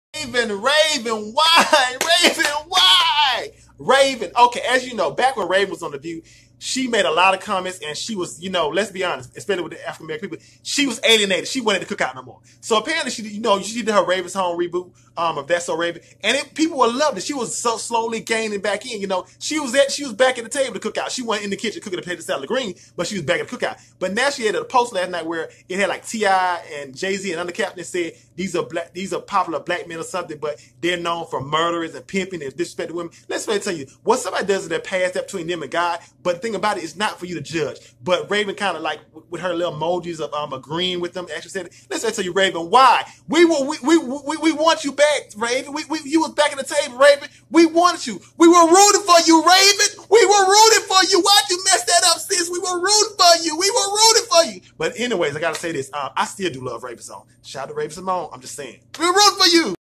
RAVEN -SYMONE GETS DRAGGED AFTER SHADING T.I, DIDDY, JAYZ, & OTHER ON INSTAGRAM ( RAVEN WHY)???
RAVEN GETS DRAGGED AFTER SHADING T.I, DIDDY, JAYZ, & OTHER ON INSTAGRAM ( RAVEN , RAVEN WHY)?